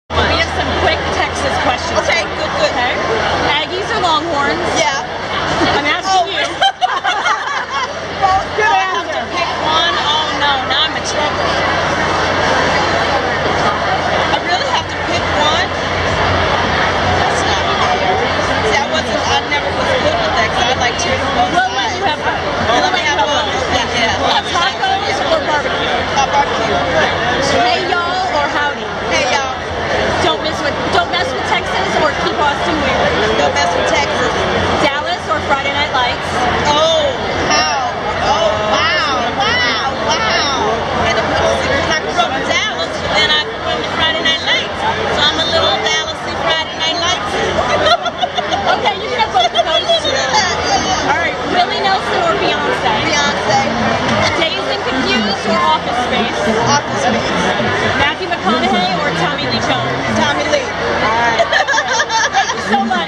Chandra Wilson at the Texas Film Awards 2016
Chandra Wilson answers our Totally Texan questions at the Texas Film Awards 2016 in Austin, Texas